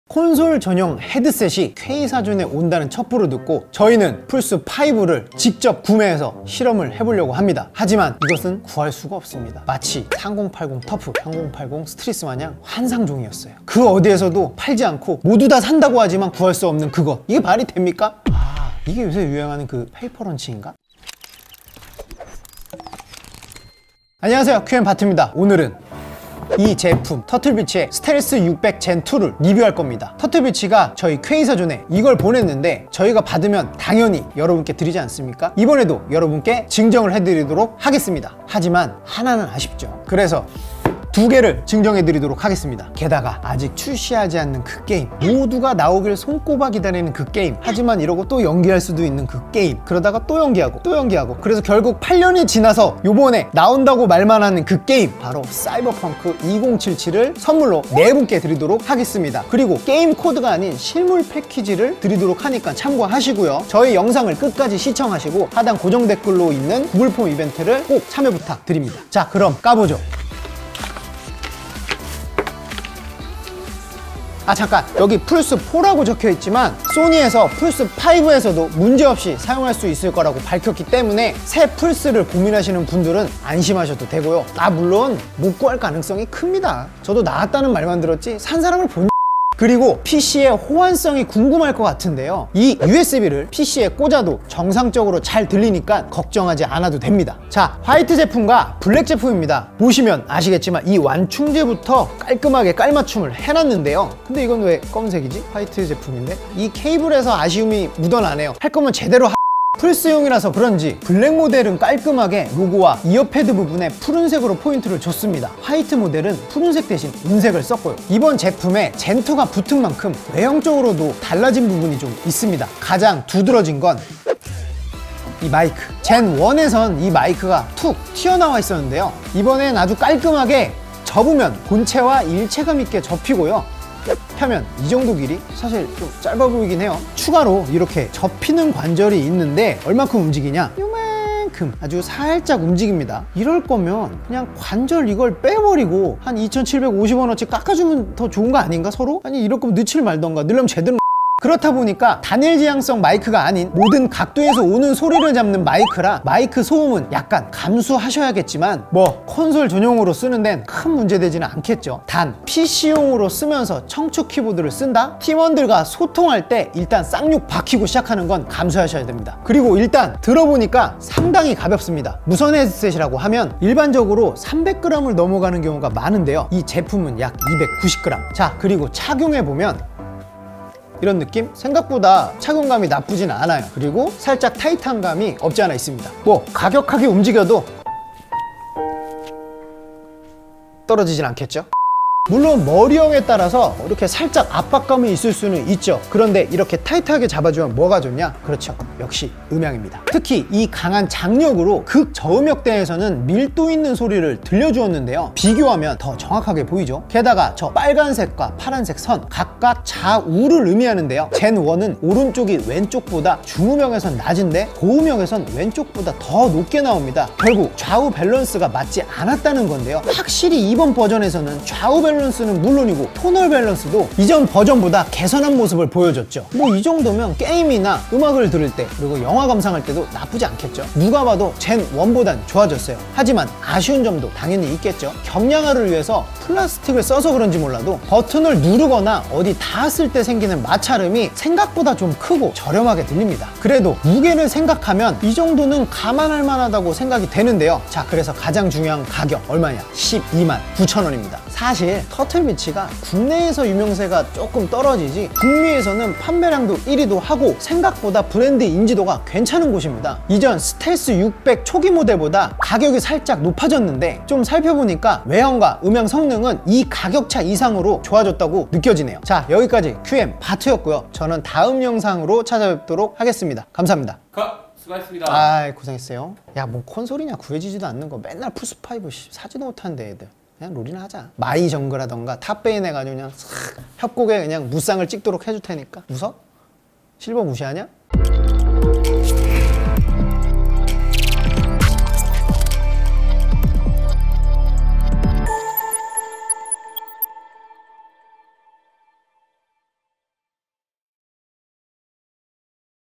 콘솔에 날개를 달아라! 터틀비치 STEALTH 600 GEN2
원래는 PS5와 같이 보여드릴려고 했는데...못 구했습니다...
하지만 신형 콘솔은 못 샀어도 헤드셋은 살 수 있잖아요 (?)
달아라! 콘솔에 날개를! 북미 1위 터틀비치 신형 헤드셋! 스텔스 600 GEN2!